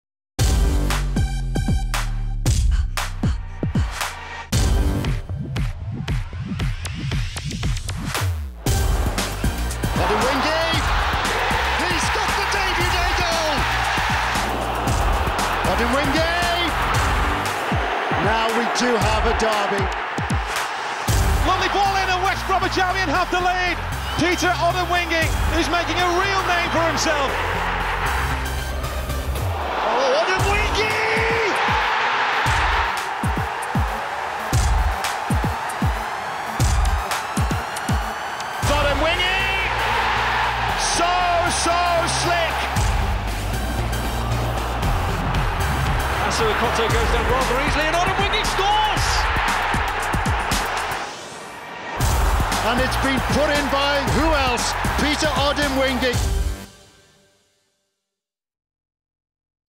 Peter Odemwingie - West Brom Saviour
Born in Uzbekistan. Raised in Russia. Plays for Nigeria. What's his name... West Brom's Peter Odemwingie.

A Premier League Live Music Piece for Setanta Sports Ireland. April 2011.

Edited by Kevin Cunnane
Produced by Brian Gleeson